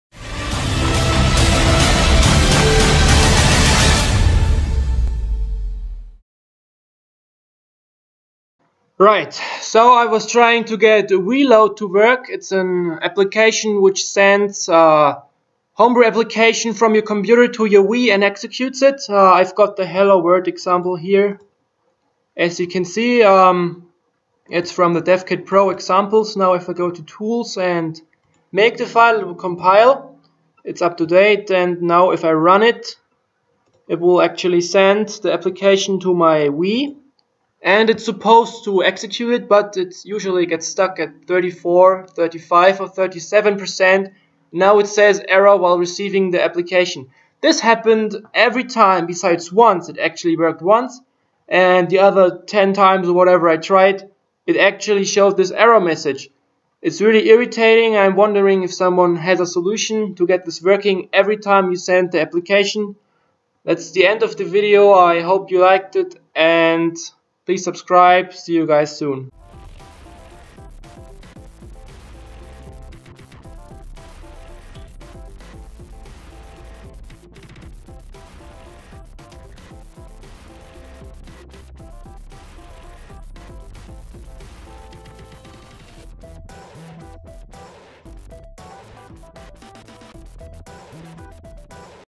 Wiiload "Error while receiving the application"
Edit:
I solved the issue myself, check it out:

Any ideas why wiiload fails to transfer the application? I'm running Windows 8 64 bit.

Note:
The link will jump to the right paragraph.